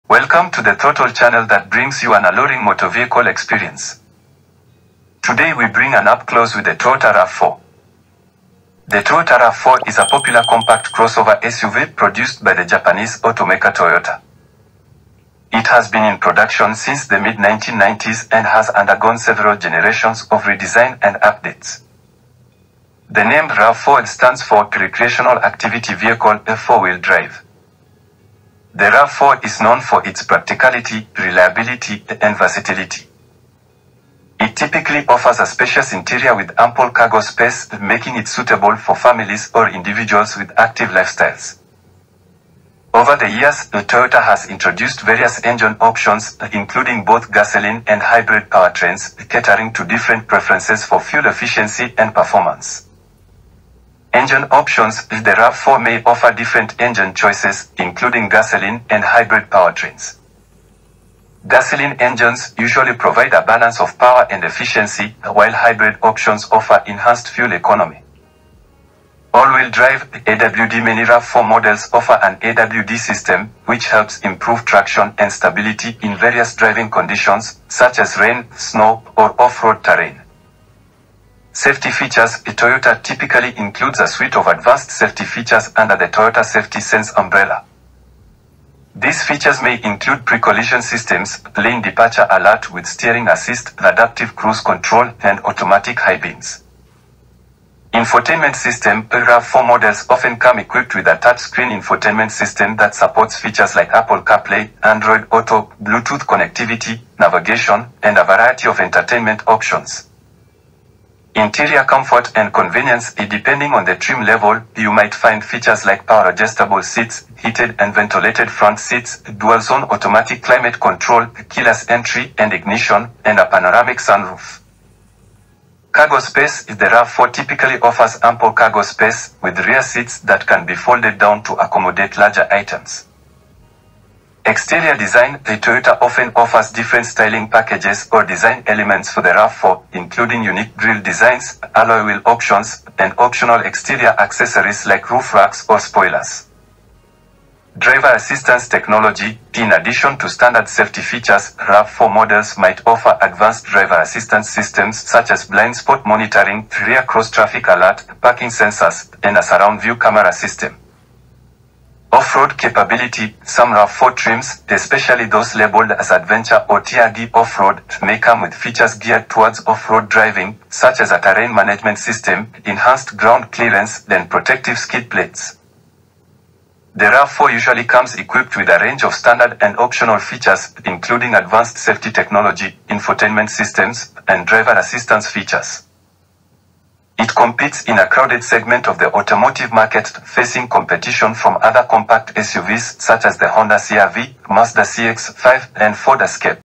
Shocking truths about the Latest TOYOTA RAV 4
Get ready to learn the shocking truths about the latest Toyota RAV4! From its features to its performance, this video will cover everything you need to know about this popular SUV.